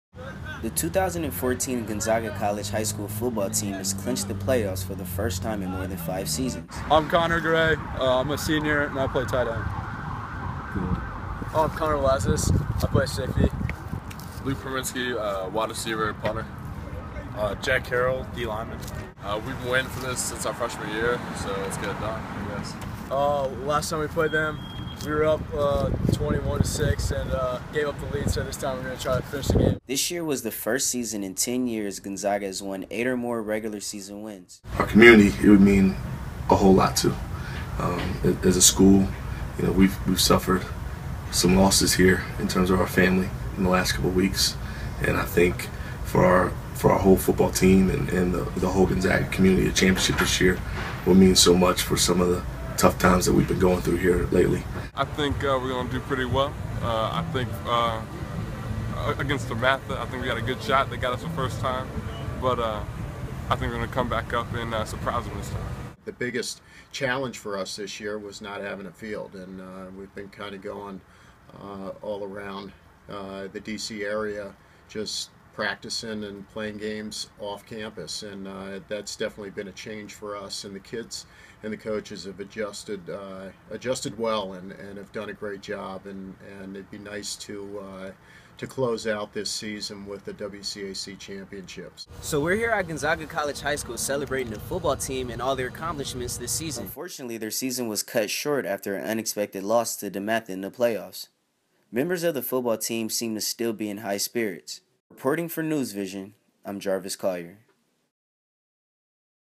Gonzaga College High School 2014 Football
The 2014 Gonzaga College High School Eagle's had a great season up until they faced DeMatha High School in the first round of the playoffs.